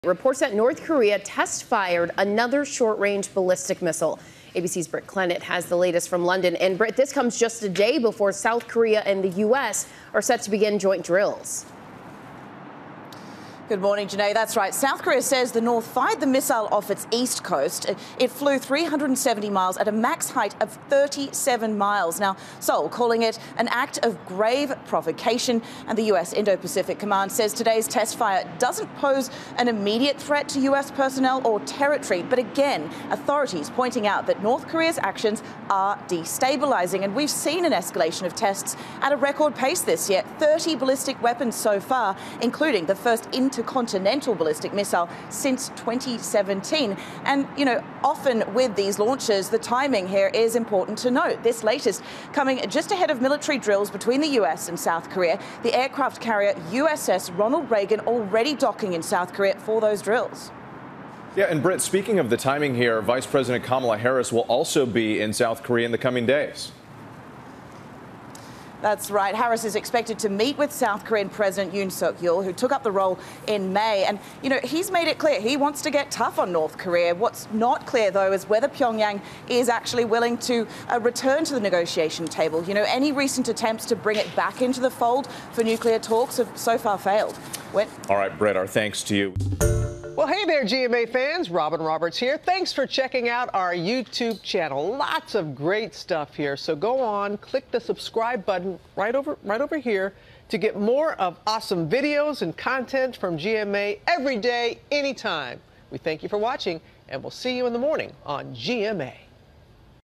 North Korea test-fires short-range ballistic missiles off its east coast
The latest ballistic missile test from North Korea comes just ahead of military drills between the U.S. and South Korea. Vice President Kamala Harris is expected to visit South Korea.